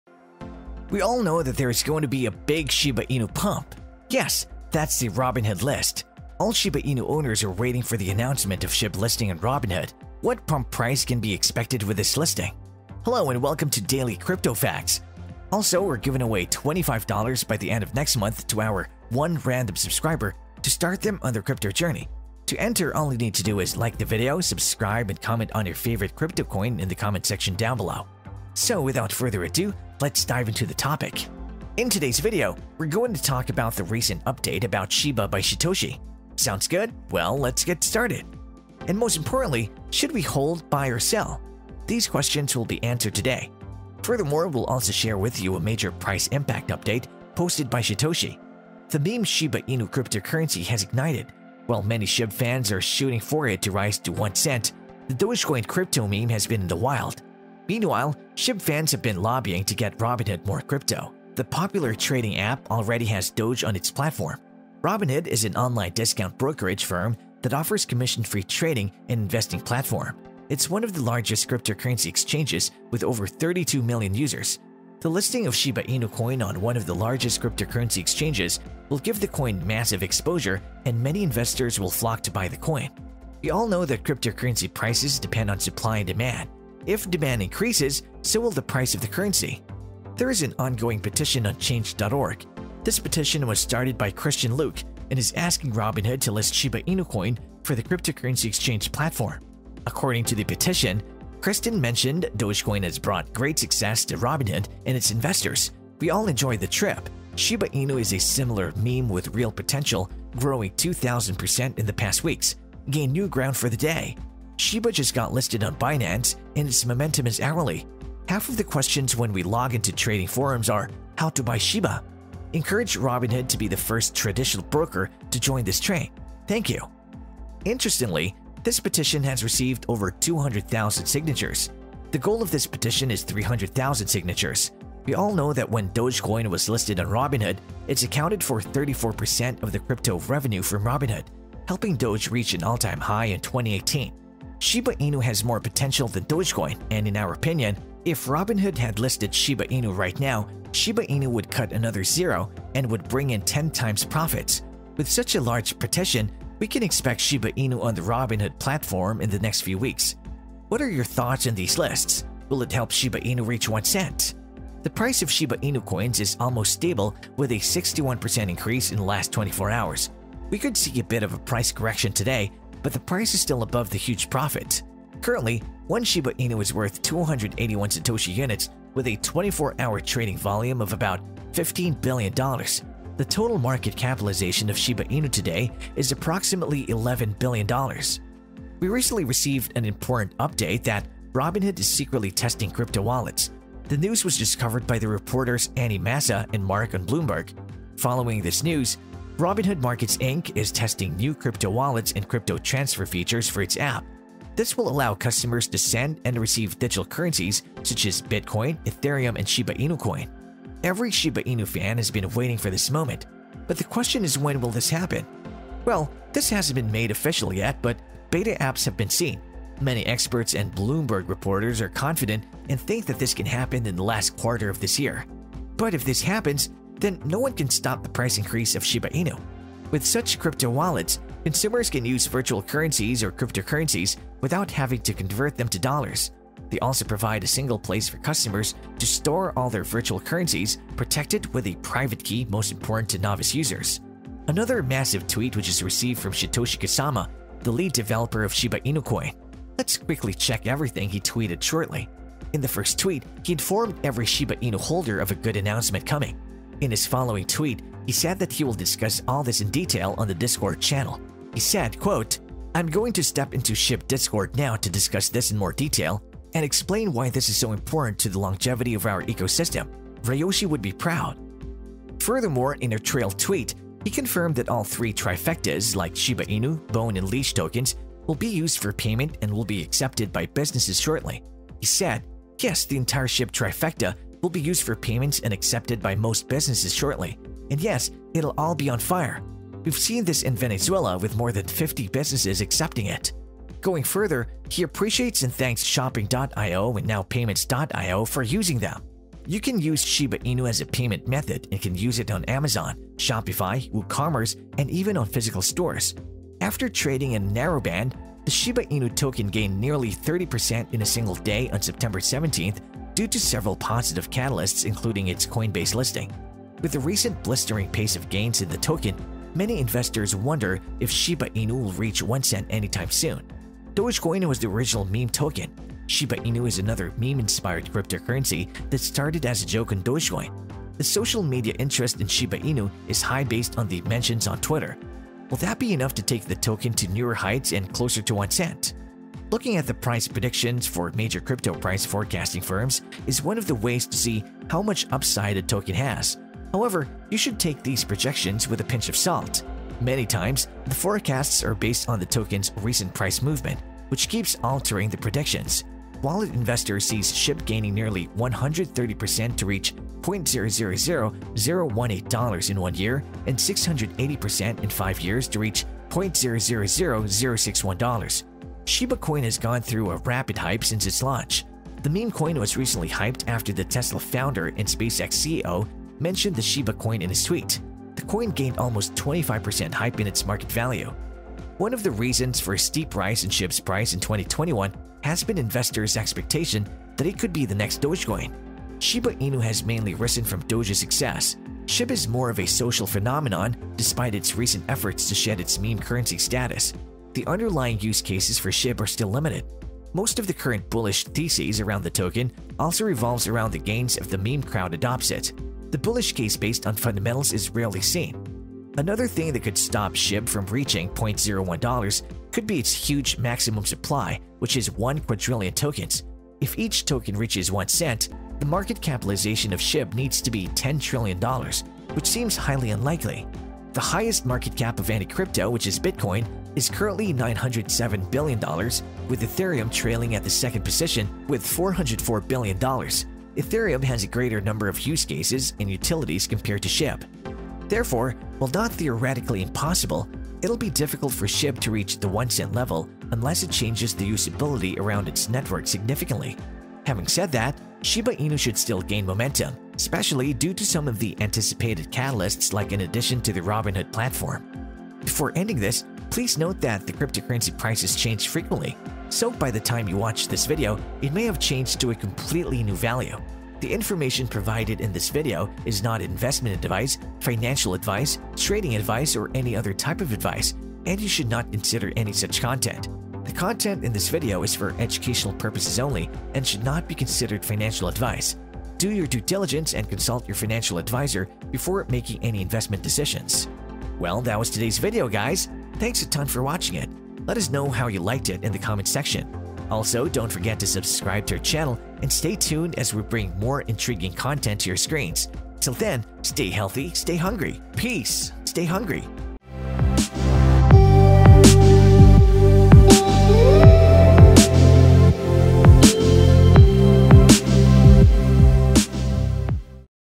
SHYTOSHI COMFIRMED MASSIVE SHIBA INU COIN BURN! SHIB TOKEN TO $0.10 BY JANUARI?!
Shytoshi confirmed  Massive shiba Inu Coin Burn | Will shib token reach $0,10 by January? The listing of Shiba Inu Coin on one of the largest cryptocurrency exchanges will give the coin massive
exposure and many investors will flock to buy the coin. We all know that cryptocurrency prices depend
on supply and demand. If demand increases, so will the price of the currency.There is an ongoing petition on change.org. This petition was started by Christin Luke and is asking
Robinhood to list Shiba Inu Coin for their cryptocurrency exchange platform. We recently received an important update that Robinhood is secretly testing crypto wallets. The news
was just covered by their reporters Annie Massa and Mark on Bloomberg. Following this news,

Robinhood Markets Inc. is testing new crypto wallets and crypto transfer features for its app. This will
allow customers to send and receive digital currencies such as Bitcoin, Etherium, and Shiba Inu Coin.
Every Shiba Inu fan has been waiting for this moment!